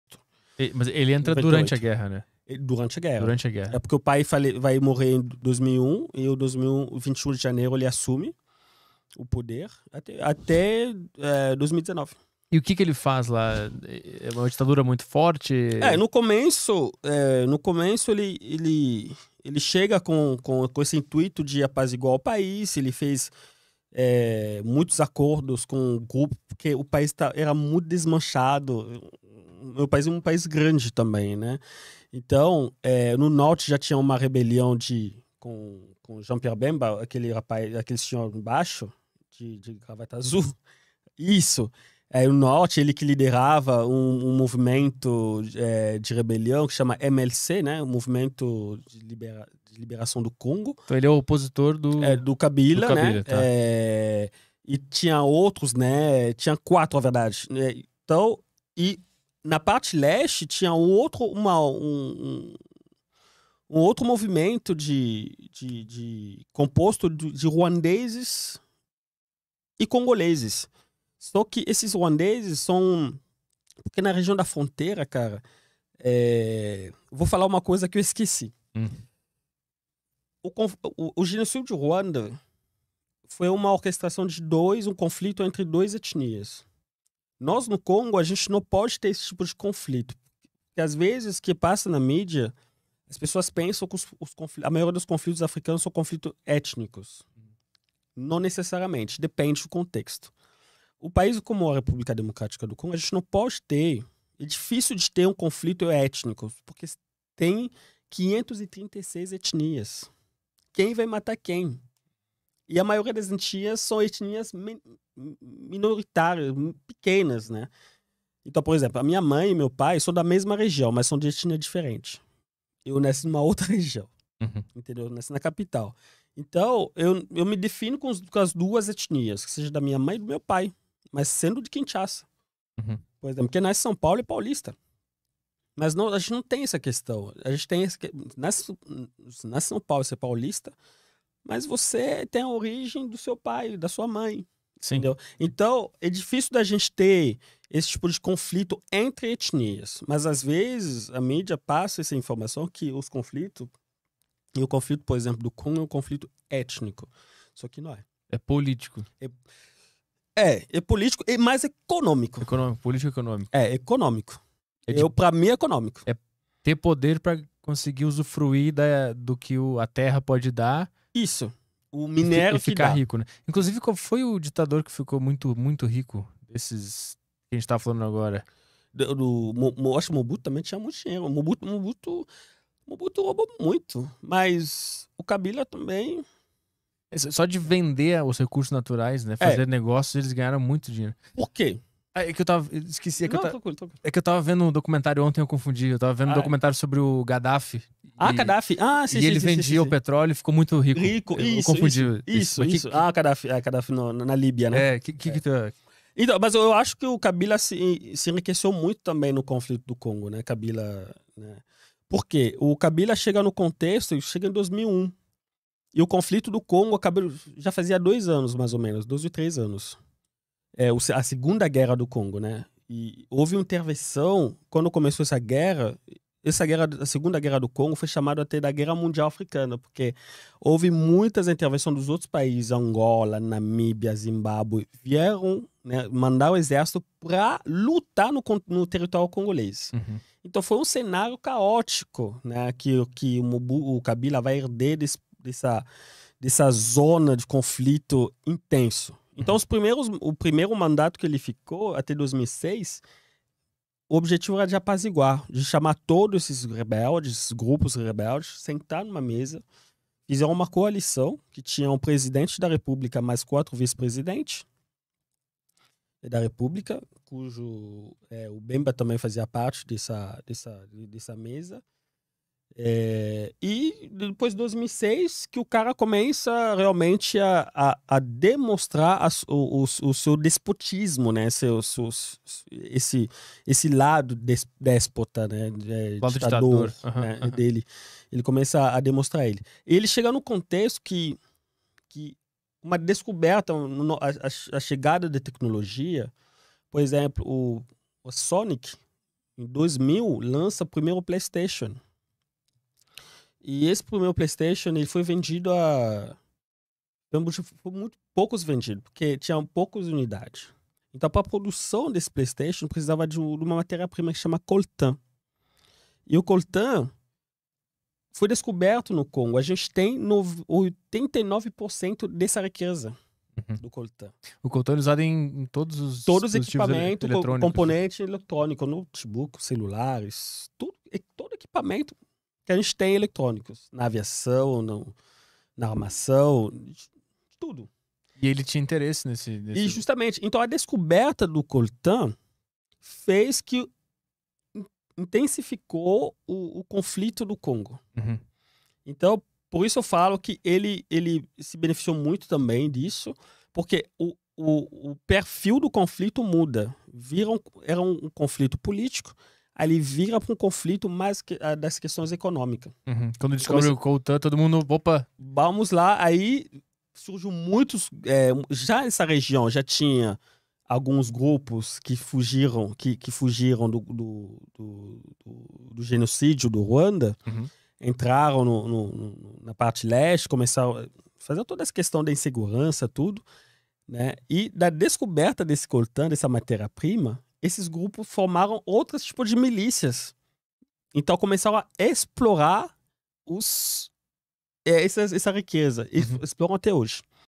Como o LANÇAMENTO do PLAYSTATION intensificou a GUERRA no CONGO | Cortes À Deriva
À Deriva é um podcast que pertence ao Flow, sendo apresentado pelo Arthur Petry.